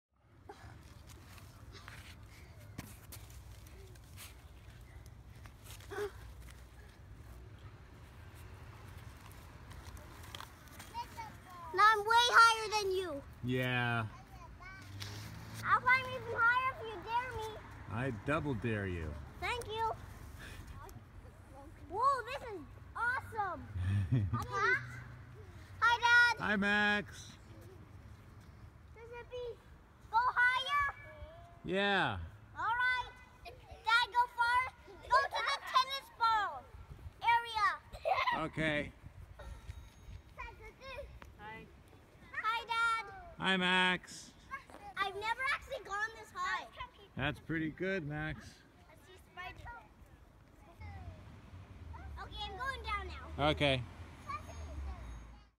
Max up a tree
Exactly what it says on the tin. Max climbs his favourite tree at Heather Park. Go Max!